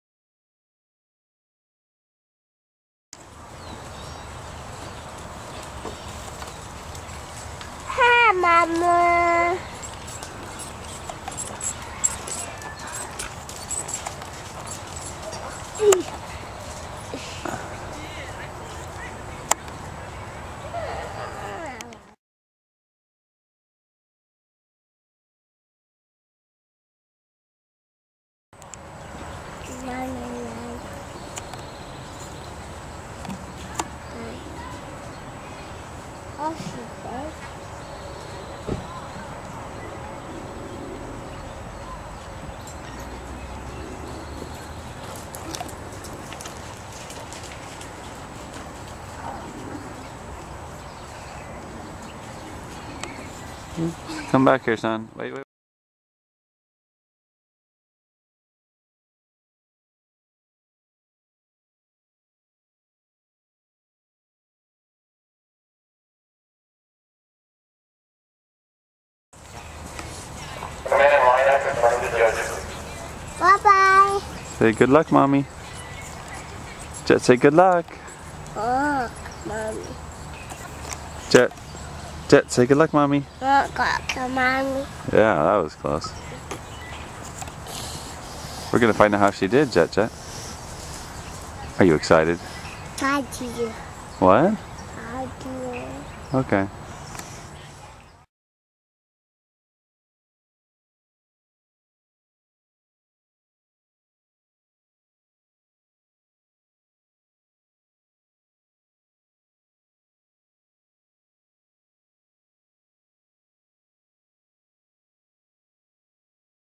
Spring Show 2008
Our second show--just four months after my second back surgery--was a huge success.  My son didn't allow for many pics or videos, and we missed out on seeing the later classes when Ana had worked out her stiffness and was moving much better.  Oh well!